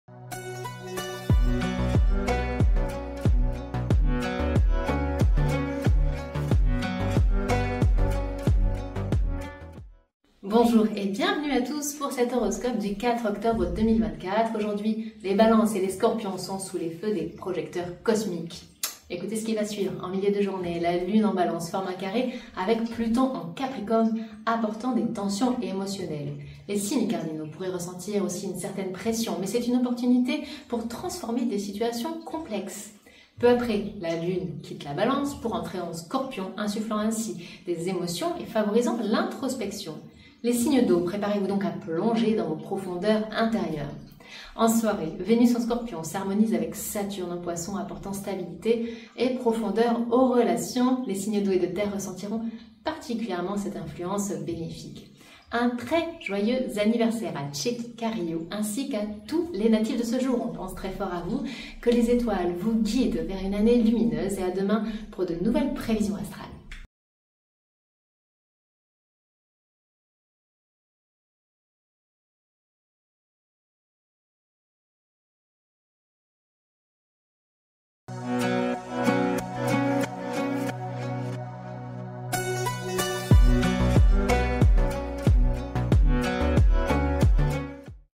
Horoscope du 4 octobre 2024 : Balance et Scorpion à l'honneur
Découvrez votre horoscope du 4 octobre 2024 avec un focus sur les Balance et Scorpion. La Lune en Balance forme un carré avec Pluton, apportant des tensions émotionnelles, avant de passer en Scorpion pour intensifier les émotions. Vénus en Scorpion s'harmonise avec Saturne, stabilisant les relations. Joyeux anniversaire à Tchéky Karyo, notre Balance du jour !

Appelez les meilleurs astrologues et voyants au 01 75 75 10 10 et profitez de 10 minutes gratuites pour les nouveaux clients!